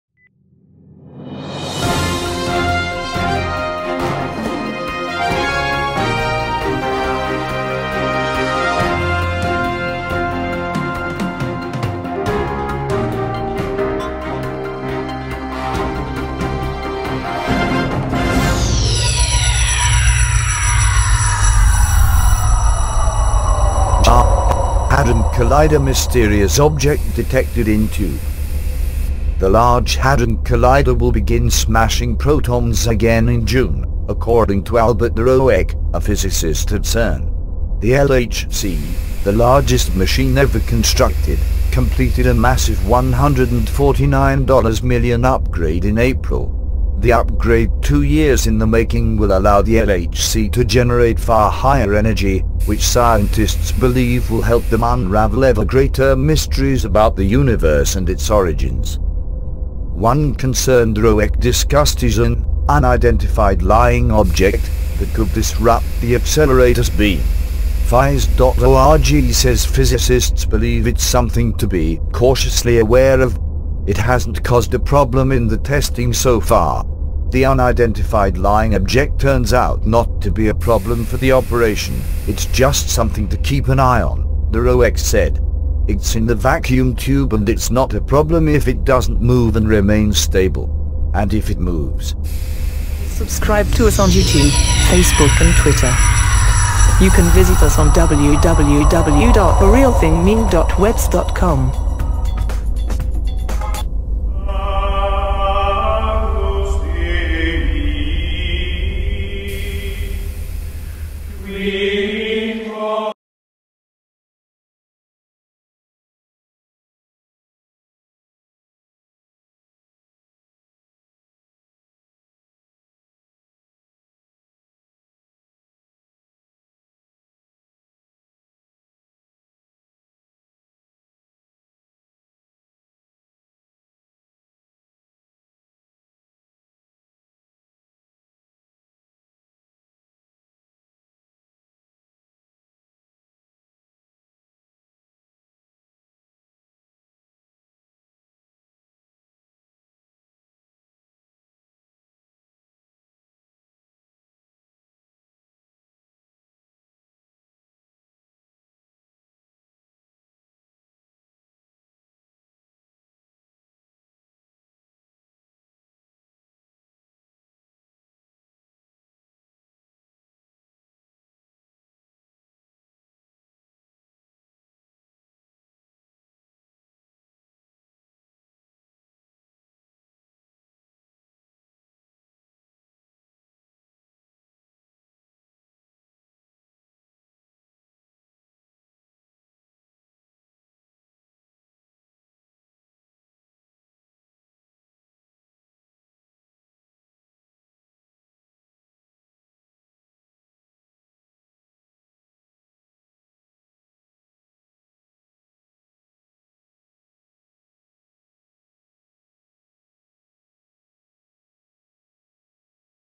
Large Hadron Collider — Mysterious Object Detected In Tube
The Large Hadron Collider will begin smashing protons again in June, according to Albert De Roeck, a physicist at CERN. The LHC, the largest machine ever constructed, completed a massive $149 million upgrade in April. The upgrade--two years in the making--will allow the LHC to generate far higher energy, which scientists believe will help them unravel ever greater mysteries about the universe and its origins.

One concern De Roeck discussed is an "unidentified lying object" that could disrupt the accelerator's beam. Phys.org says physicists believe it's something to be "cautiously aware of." It hasn't caused a problem in the testing so far. "The unidentified lying object turns out not to be a problem for the operation, it's just something to keep an eye on," De Roeck said. "It's in the vacuum tube and it's not a problem if it doesn't move and remains stable." And if it moves?